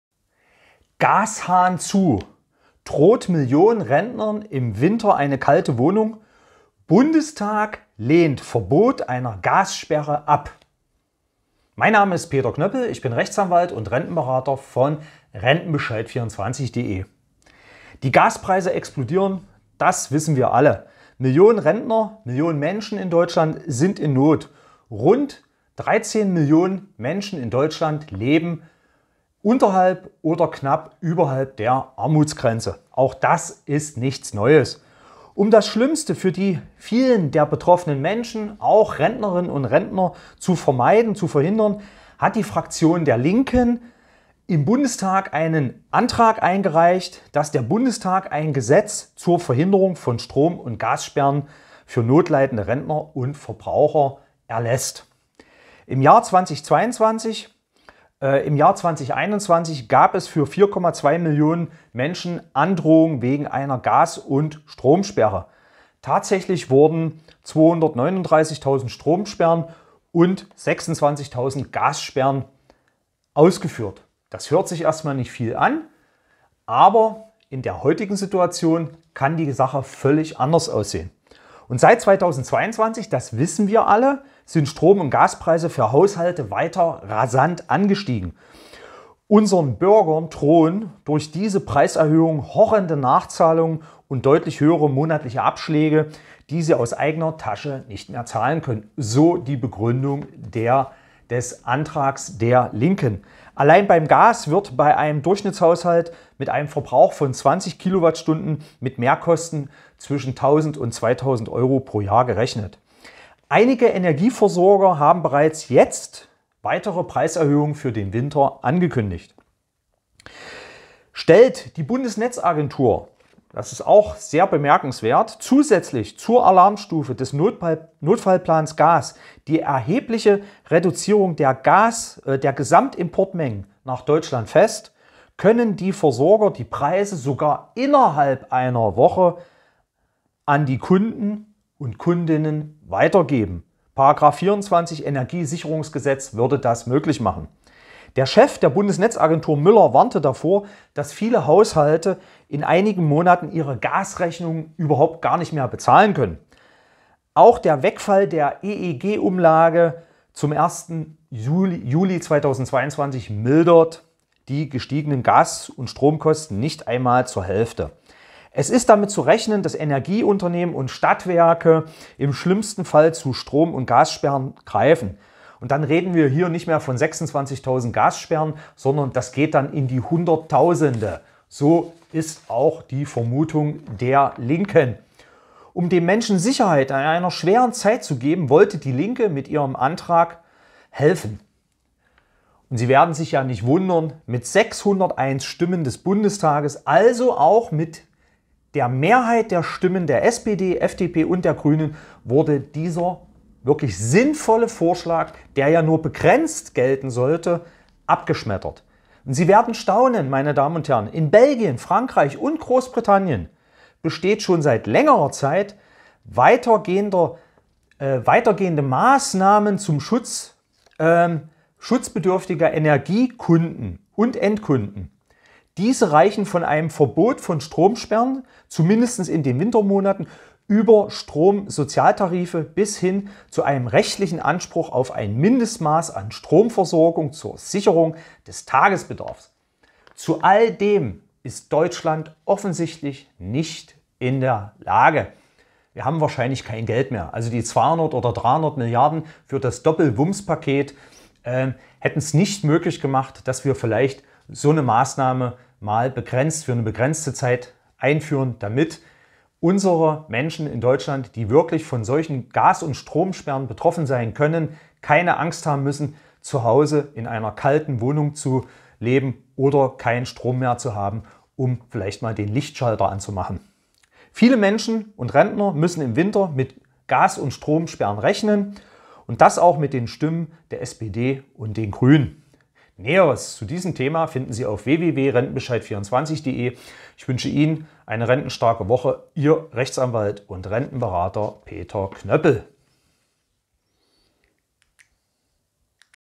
Herzloser Bundestag: müssen Millionen arme Rentner wegen Gassperre im Winter frieren?
Horrende Gas-und Stromrechnungen drohen. Millionen Menschen wissen nicht mehr ein noch aus!
Der Antrag der  Partei der Linken ein zeitlich befristetes Verbot von  Strom-und Gassperren für Millionen Menschen und Rentner in Deutschland durch eine Bundestagsgesetz zu erlassen, wurde mit der Mehrheit der stimmen des Bundestages abgelehnt. Darunter auch mit den Stimmen der SPD und den Grünen, die sich die soziale Gerechtigkeit, den Kampf gegen Altersarmut und den Schutz der Armen und Schwachen in unserem Land auf die "Fahne" geschrieben haben. Diese Entscheidung ist nicht zuverstehen.  Länder wie Frankreich-England haben Sozialtarife und Verbot von Strom-und Gassperrren eingeführt- wahrlich kein Sozialismus!